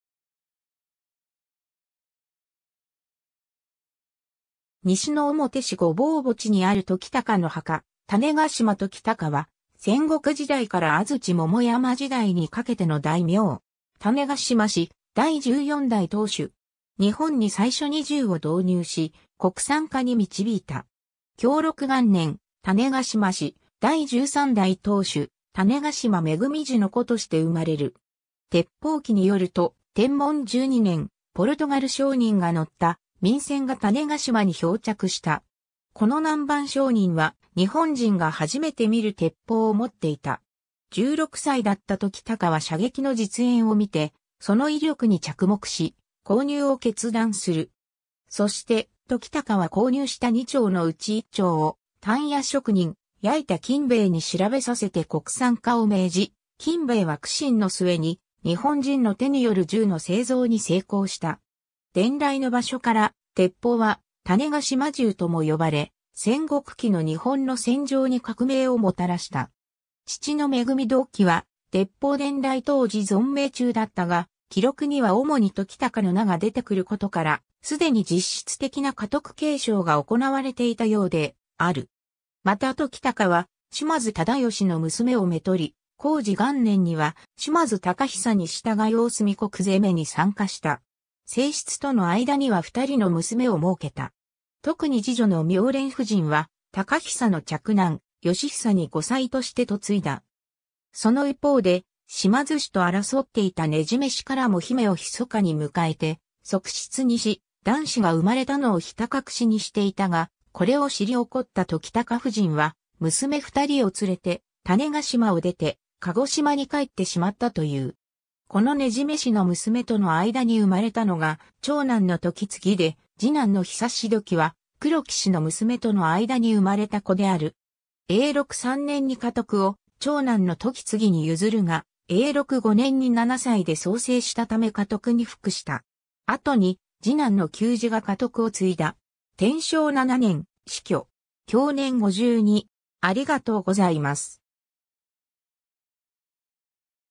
種子島時尭
西之表市御坊墓地（種子島氏最初の墓地）にある時堯の墓（中央） 種子島時堯（たねがしま ときたか）は、戦国時代から安土桃山時代にかけての大名。
種子島氏第14代島主。
日本に最初に銃を導入し、国産化に導いた。
 享禄元年（1528年）、種子島氏第13代島主・種子島恵時の子として生まれる。
 『鉄炮記』によると、天文12年（1543年）、ポルトガル商人が乗った明船が種子島に漂着した。
この南蛮商人は、日本人がはじめて見る鉄砲を持っていた。
16歳だった時堯は射撃の実演を見てその威力に着目し購入を決断する。
そして時堯は購入した二挺のうち一挺を鍛冶職人八板金兵衛に調べさせて国産化を命じ、金兵衛は苦心の末に、日本人の手による銃の製造に成功した（もう一挺は島津氏を通して、室町幕府将軍足利義晴に献上）。
伝来の場所から鉄炮は種子島銃とも呼ばれ、戦国期の日本の戦場に革命をもたらした。
 父の恵時は鉄砲伝来当時存命中だったが、記録には主に時堯の名が出てくることから、既に実質的な家督継承が行われていたようである。
また時堯は、島津忠良の娘を娶り、弘治元年（1555年）には島津貴久に従い大隅国攻めに参加した。
正室との間には2人の娘を儲けた。
特に次女の妙蓮夫人（円信院殿）は、貴久の嫡男・義久に後妻として嫁いだ。
その一方で、島津氏と争っていた禰寝氏からも姫を密かに迎えて側室にし、男子が生まれたのをひた隠しにしていたが、これを知り怒った時堯夫人は、娘2人を連れて種子島を出て鹿児島に帰ってしまったという（その後、夫人は肝付兼盛に再嫁する）。
この禰寝氏の娘との間に生まれたのが長男の時次で、次男の久時は黒木氏の娘との間に生まれた子である。
 永禄3年（1560年）に家督を長男の時次に譲るが、永禄5年（1562年）に7歳で早世したため家督に復した。
後に次男の久時が家督を継いだ。
 天正7年（1579年）、死去。
享年52。